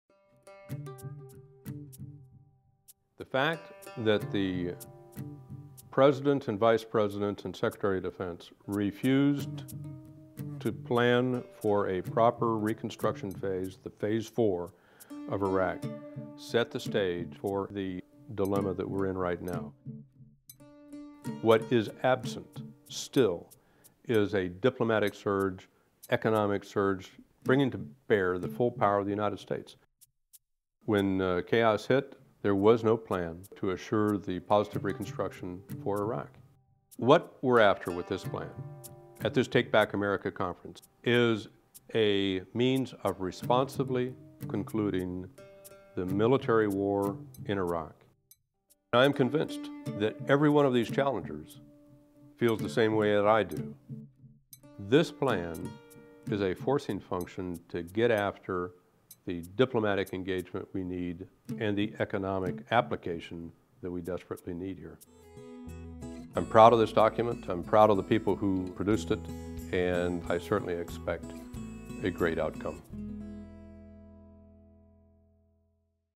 General Eaton for Take Back America Conference
Maj. General Paul D. Eaton (ret), the former Commanding General in Iraq discussing Darcy Burner's plan to responsibly end the war in Iraq.